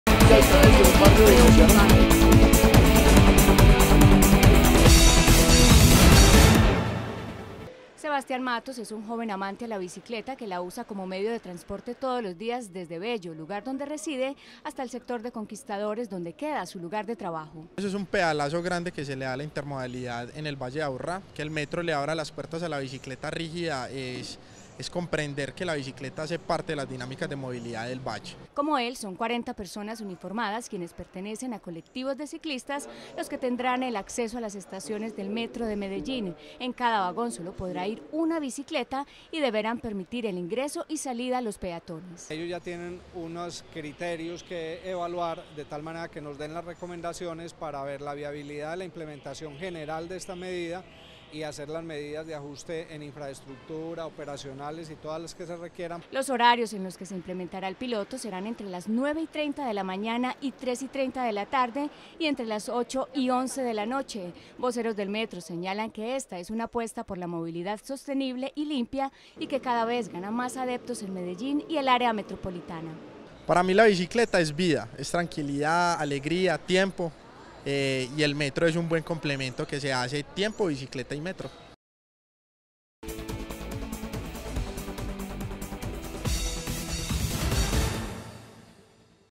El Metro comenzó el piloto para llevar bicicletas en sus vagones
El Metro de Medellín comenzó el piloto que permitirá el uso de la bicicleta convencional en sus vagones.

“Mi Bici y Yo”, es la iniciativa que va hasta el 1° de julio y que busca la viabilidad para que se implemente de manera definitiva el traslado de ciclas en el sistema masivo de transporte de la ciudad.